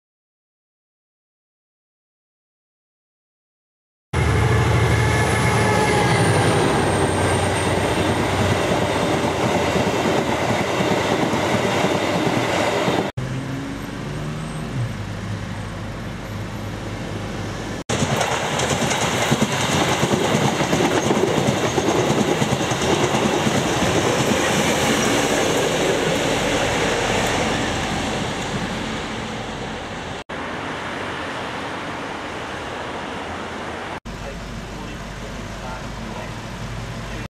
Diesels under the wires
Quick video with the electrification process out in West Auckland, New Zealand
The catenary wires are not complete yet, and nor are the traction wires.
DF7200 leaving Henderson Station and DC 4916 arriving at Henderson station.

4 April 2012

Video # 601